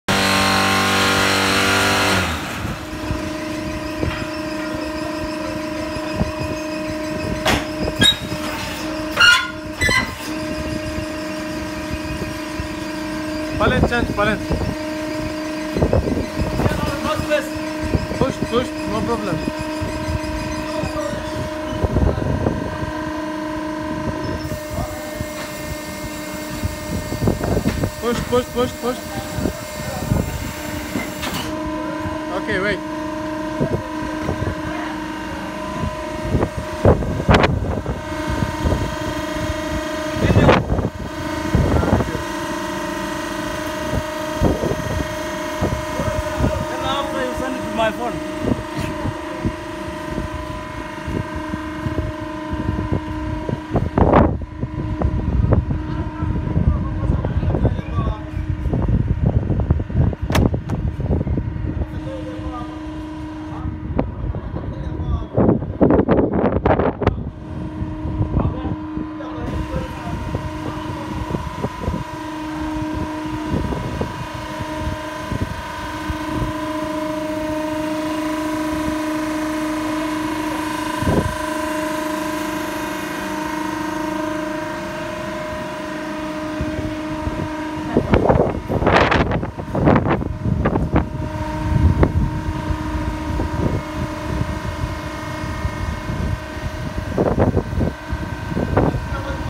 Conmach BlockKing-12MS Block and Interlock Making Machine during Double T type Interlock Production
Conmach BlockKing-12MS Block and Interlock Making Machine is in use at Kismayo Somalia. The machine is fed by a single shaft mixer through the transfer belt. The pallet pick up is made by the trolleys or man power. It produces 4 units/press of 20 cm (8 inch) block, 5 units/press of 15 cm (6 inch) block and 7 units/press of 10 cm (4 inch) block in one press and also ability to produce 0,325 m2/press interlocking paver